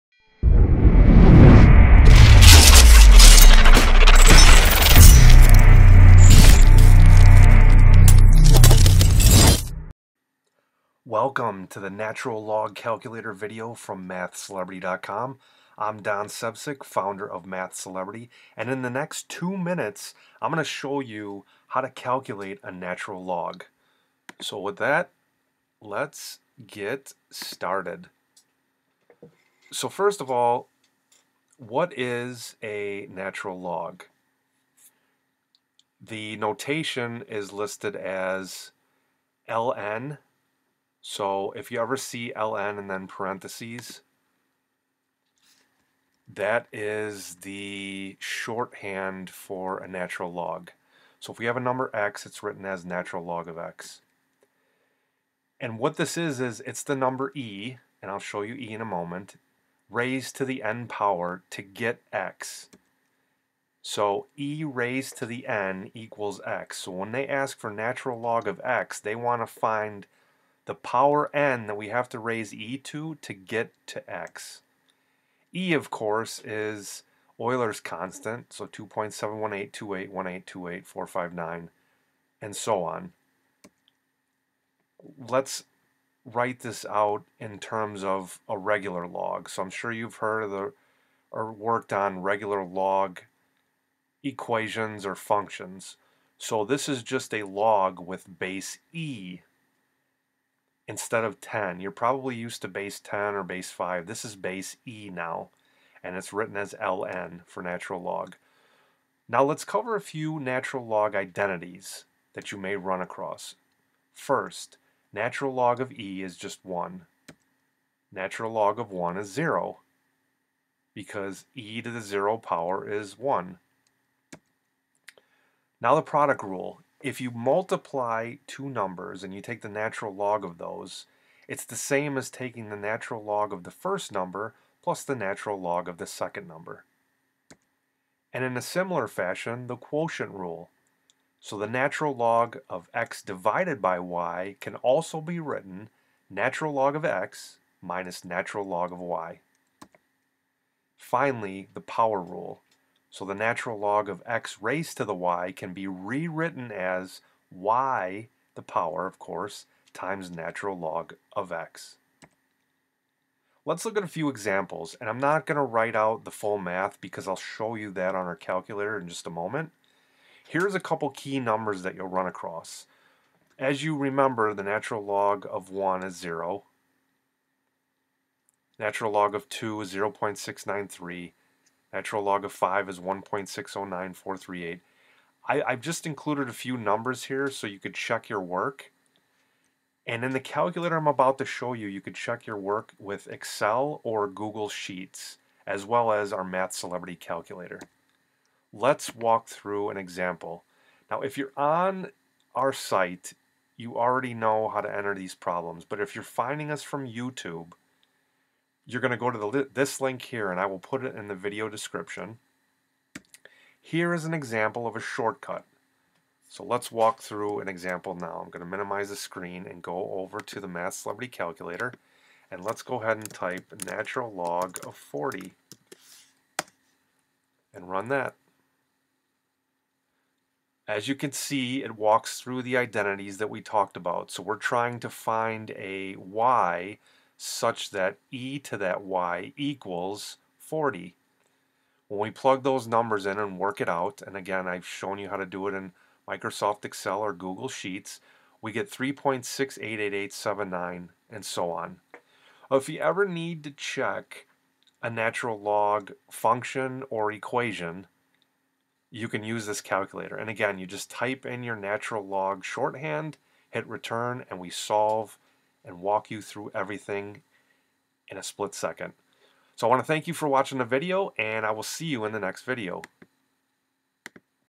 Natural Log Calculator
Walks you through the natural log function in pre-calculus, including the identities such as:
Product Rule, Quotient Rule, Power Rule using our Natural Log Calculator. Get the tablet and products I use for math here: